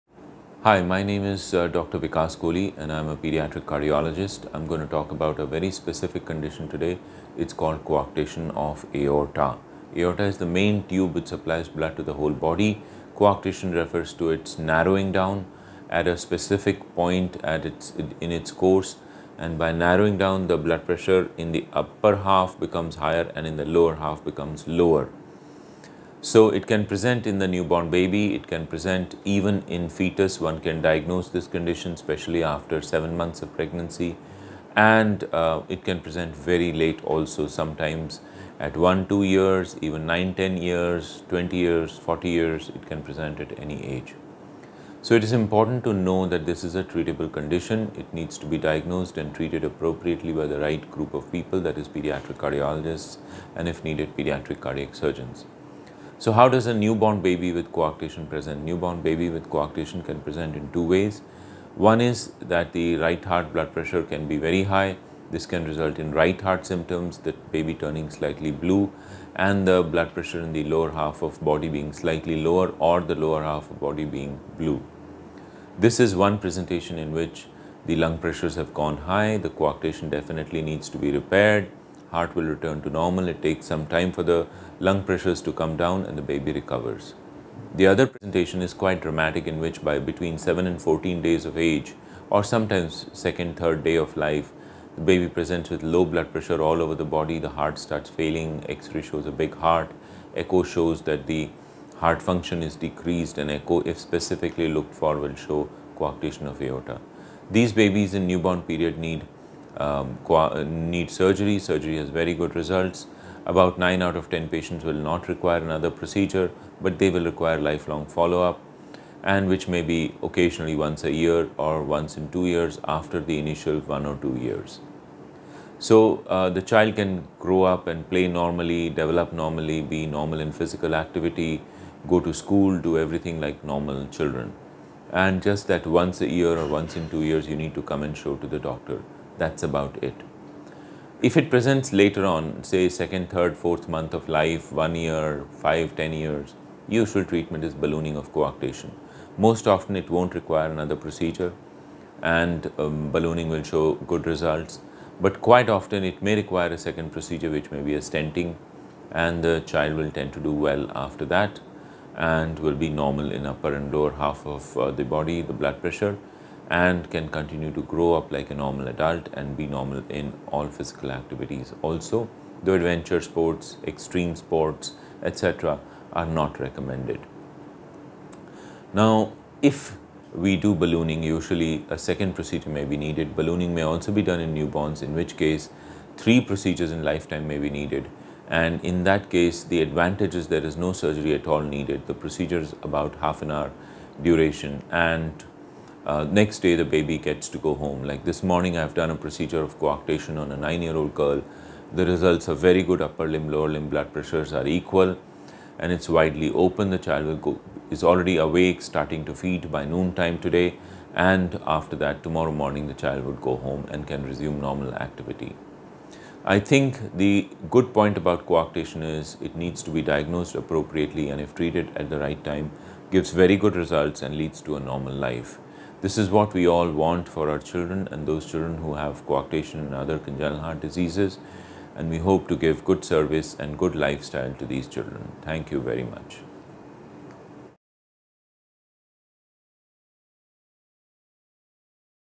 COARCTATION OF THE AORTA || COA || DR. VIKAS KOHLI || PEDIATRIC CARDIOLOGIST || BALLOONING
Topic: Coarctation of Aorta (COA)
Expert: Dr. Vikas Kohli (Pediatric Cardiologist)

As a part of our initiative of educating the masses about different types of Congenital Heart Defects commonly known as CHD in a simple way, we are sharing a video on Coarctation of the Aorta (COA) by our expert Dr. Vikas Kohli. Two types of treatment options and footage of procedure video.